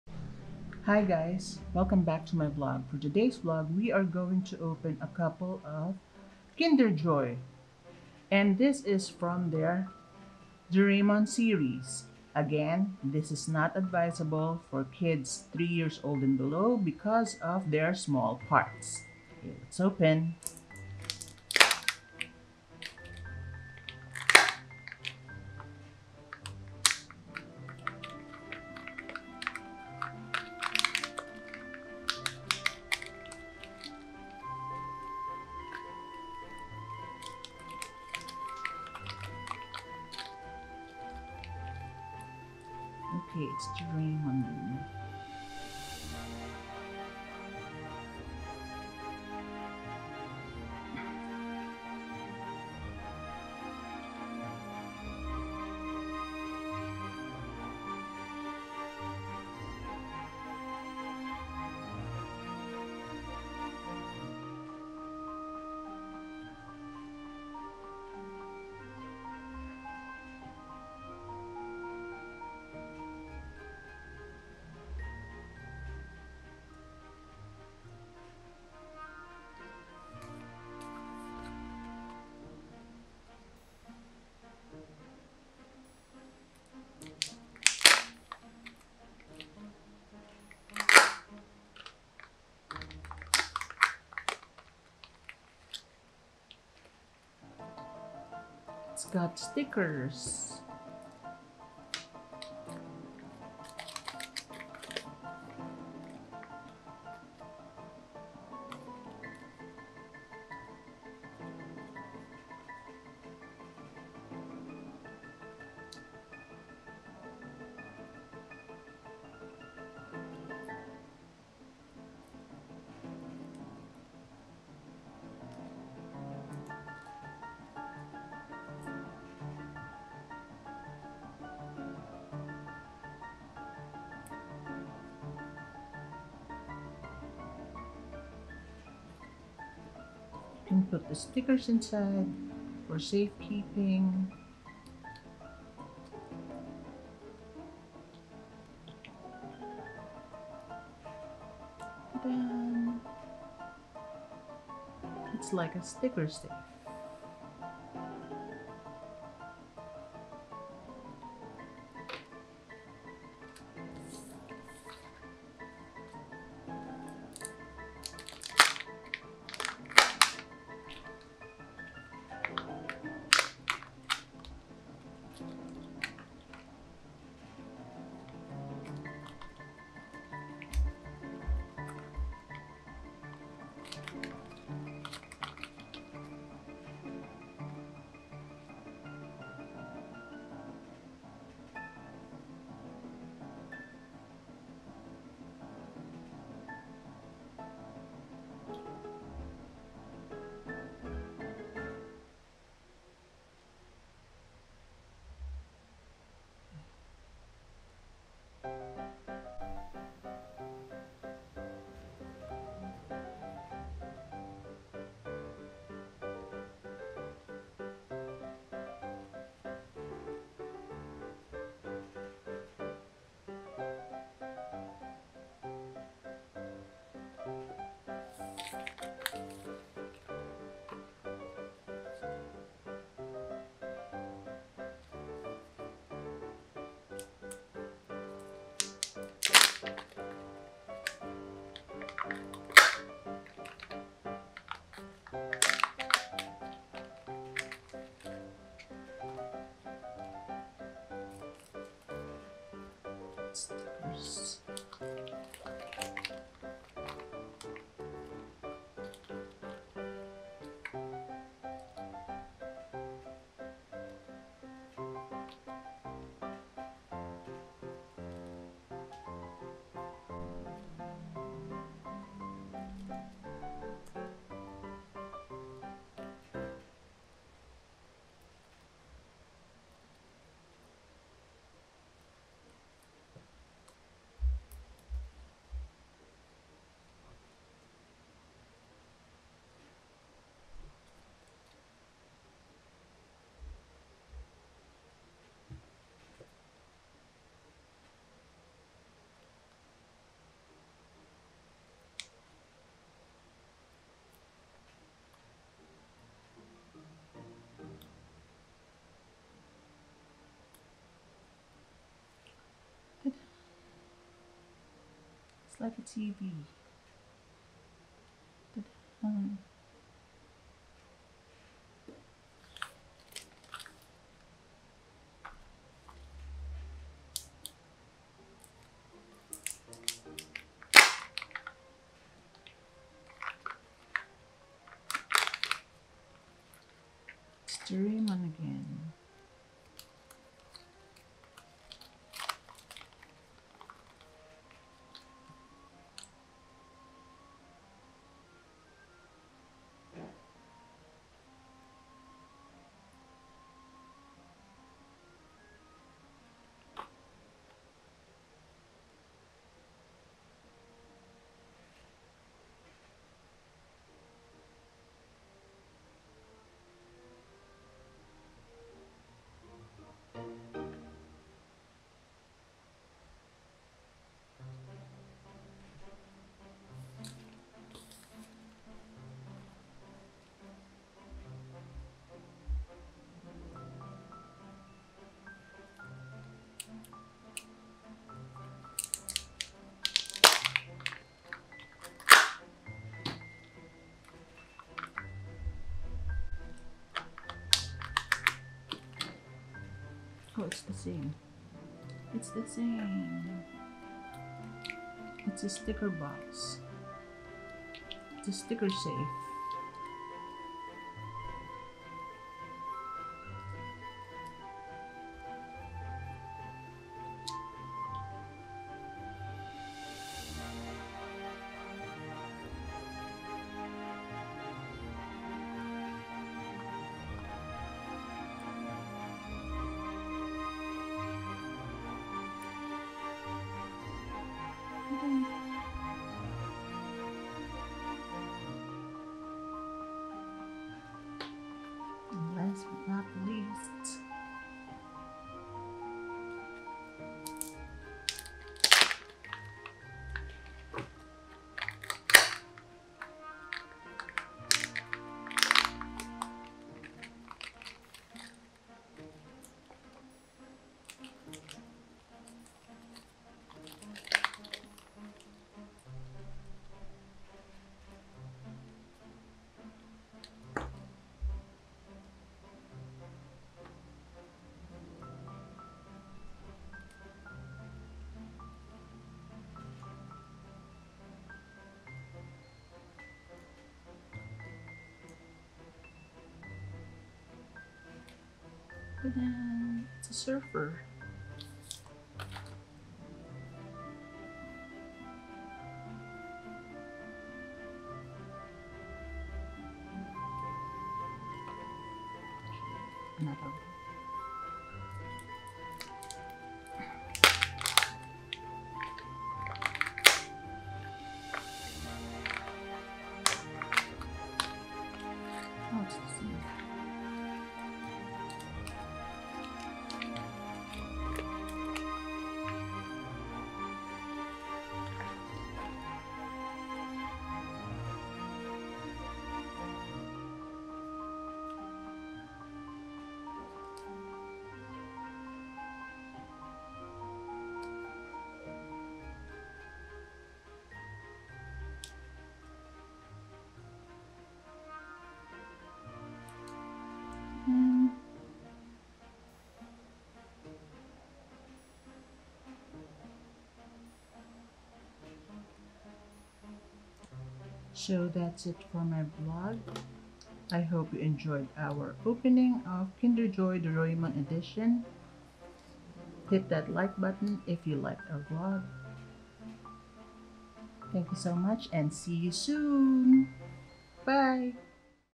KINDER JOY:DORAEMON
KINDER JOY:DORAEMON (Episode 3)
Hi guys its another Kinder Joy day once again. and today we are going to unbox/hatch several Kinder Joy Doraemon series... Enjoy. And God Bless

such as criticism, comment, news reporting, scholarship, and research.